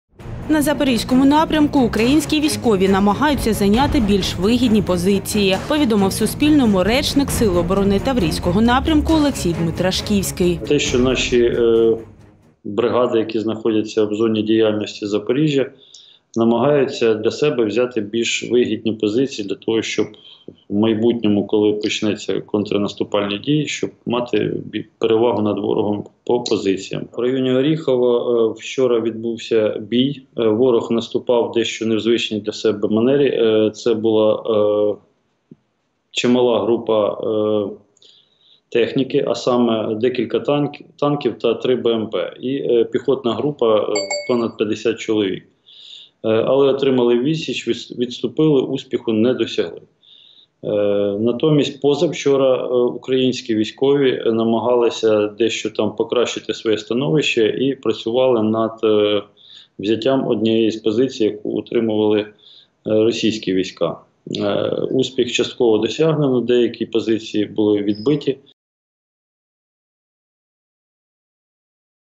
На Запорізькому напрямку українські військові намагаються зайняти більш вигідні позиції | Новини
На Запорізькому напрямку українські військові намагаються зайняти  більш вигідні позиції – повідомив Суспільному речник Сил оборони Таврійського напрямку Олексій Дмитрашківський.